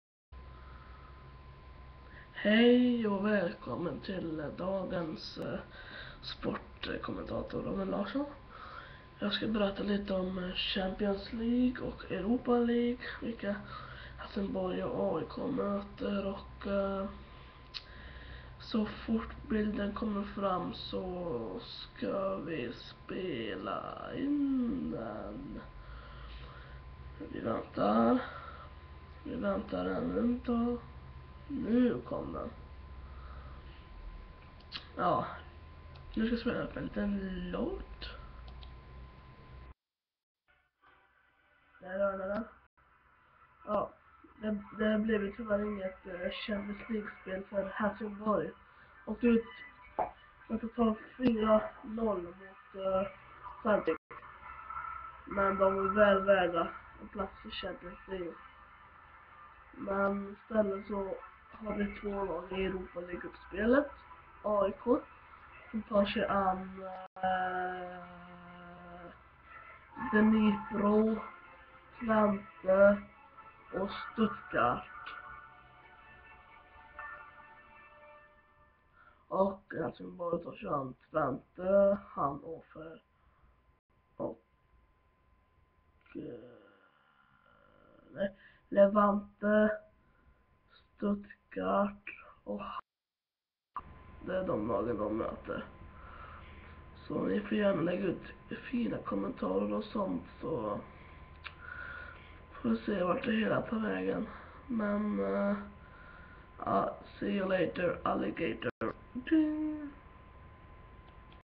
europa league
berättar grupperna i europa league